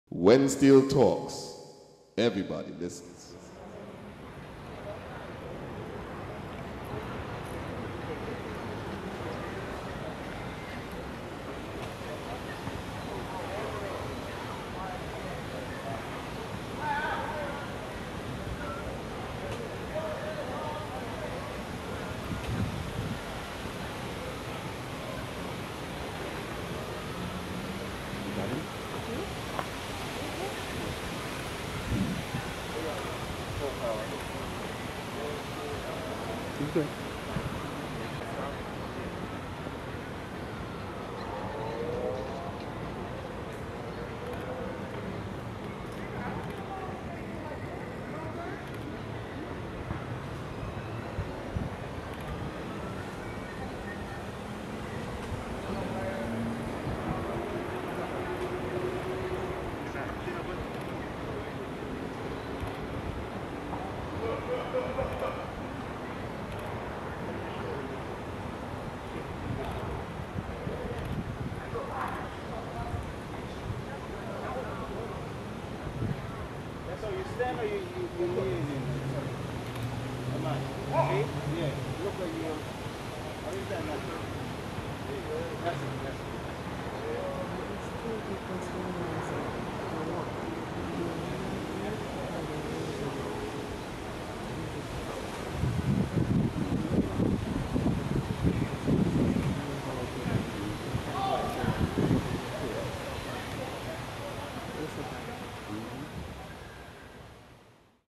crossfire photoshoot
Renowned photographer Wayne Lawrence conducts photoshoot with CrossFire Steel Orchestra for upcoming National Geographic special.